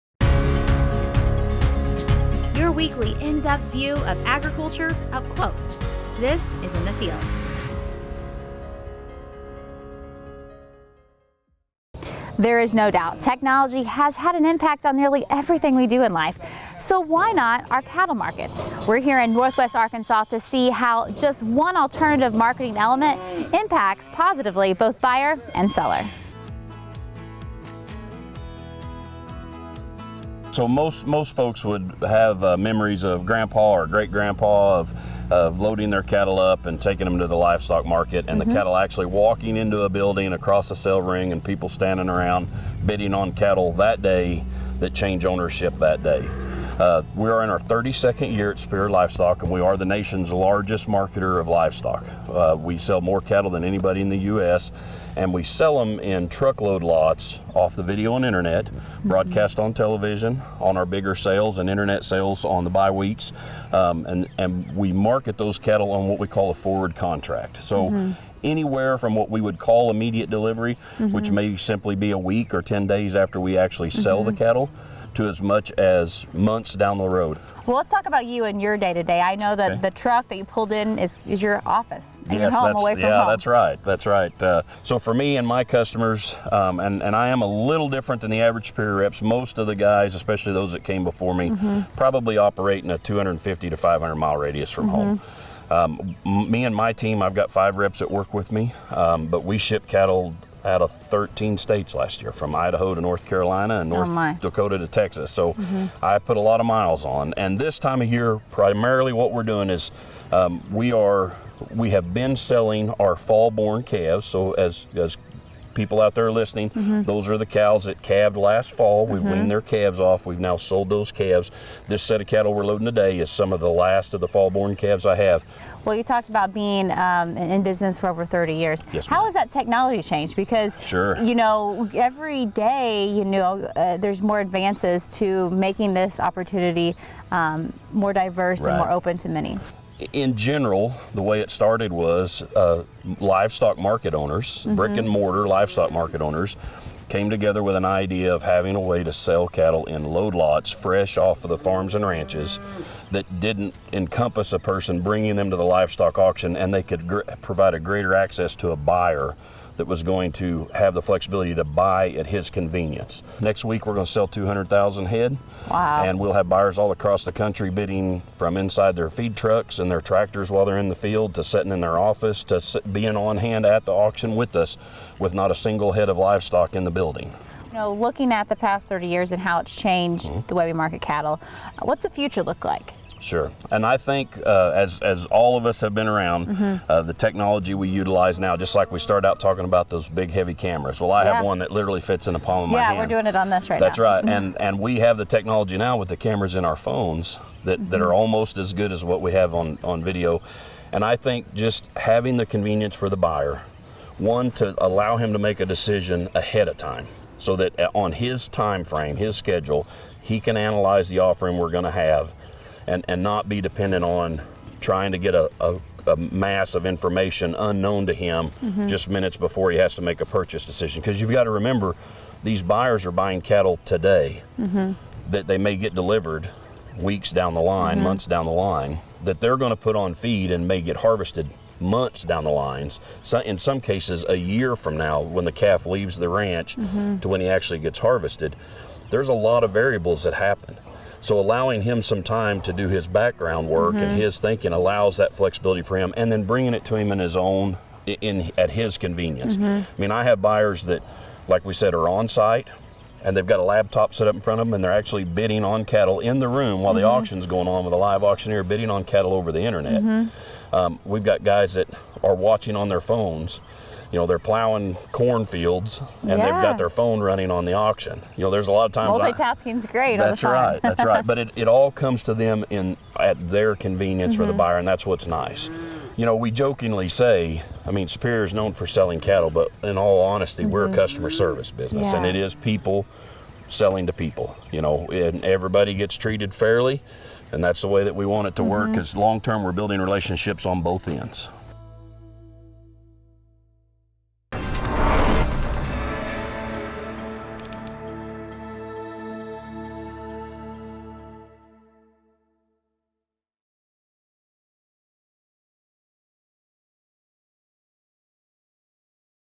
Marketing Livestock with Clint Berry & Superior Livestock
Technology has had a significant impact on nearly everything we do in life. So, why not our cattle markets? We traveled to Northwest Arkansas to see how just one alternative marketing element impacts both buyer and seller positively.